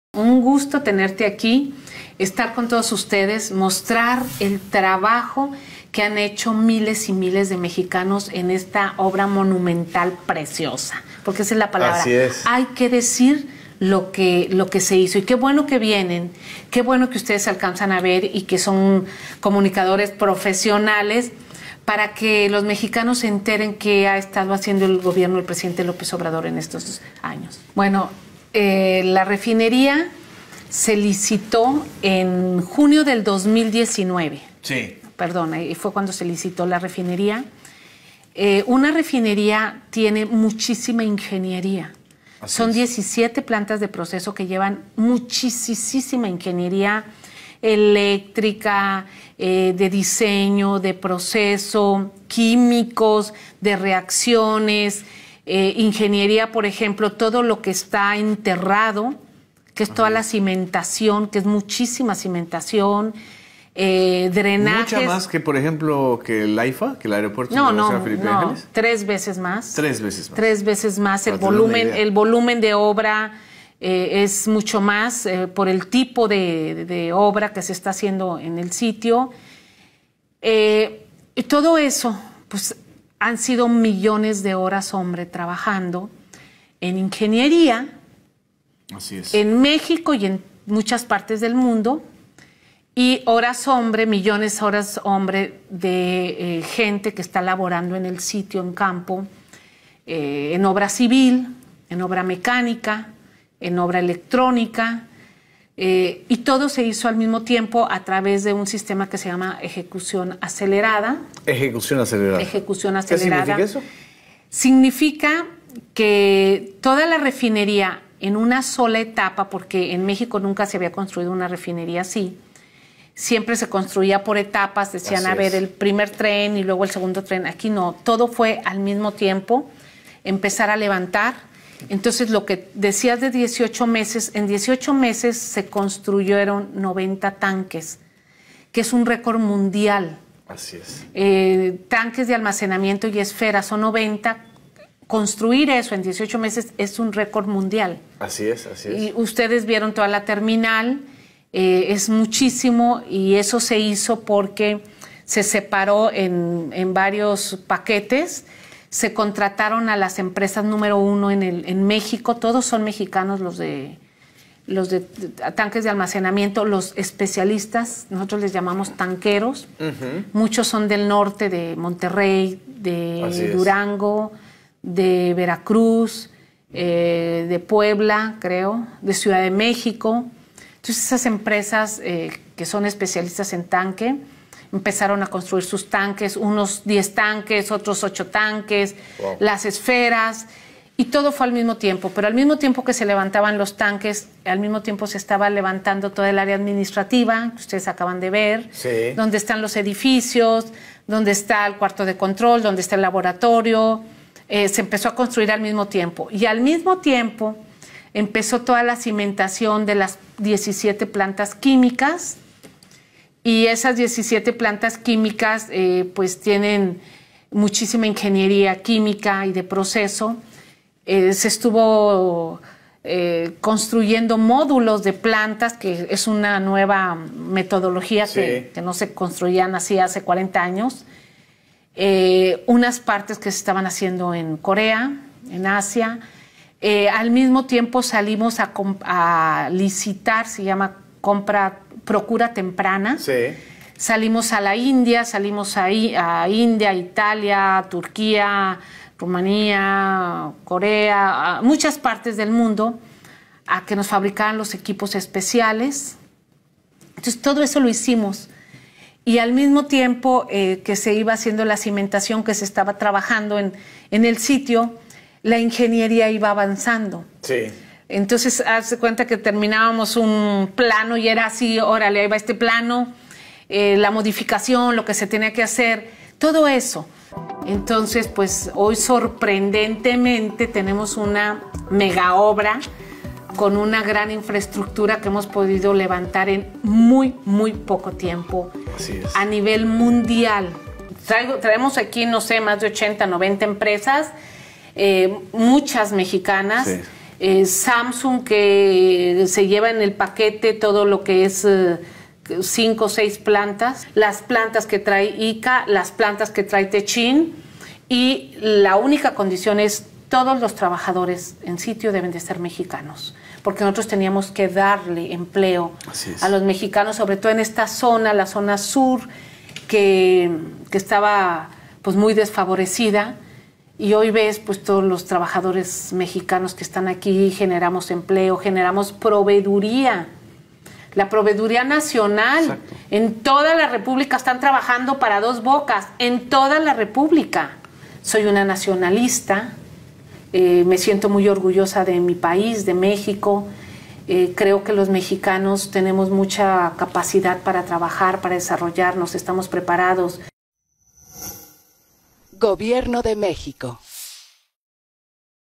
Obra Monumental en tiempo Récord Graciass los Mexicanos
En entrevista Rocío Nahle nos habla sobre la Obra Monumental construida en tiempo Record